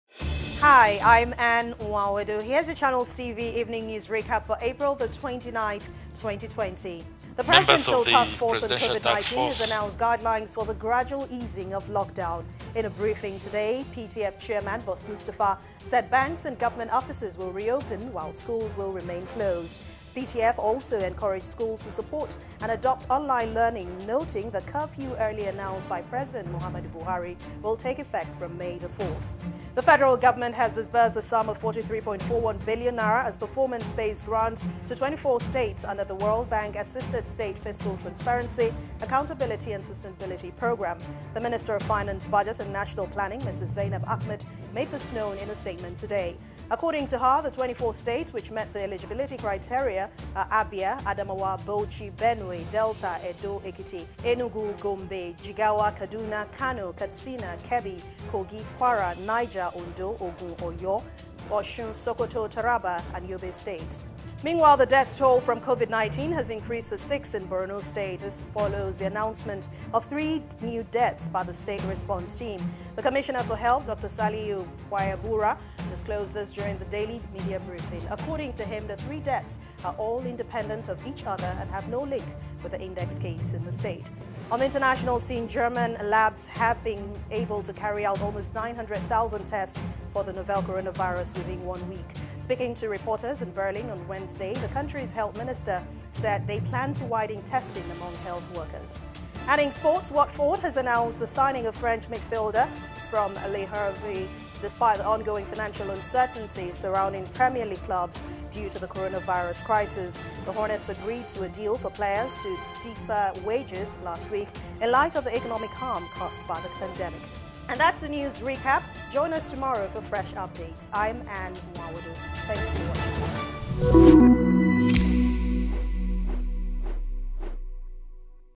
Recap: PTF Announces Guidelines For Easing Lockdown
Evening News Recap For April 29th, 2020

Start your morning updated and end the evenings with a recap of the day's headlines.

All in two minutes.